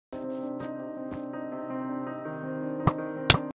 Who bridges like this?🤣
How did I not see this coming. I was chasing someone.😂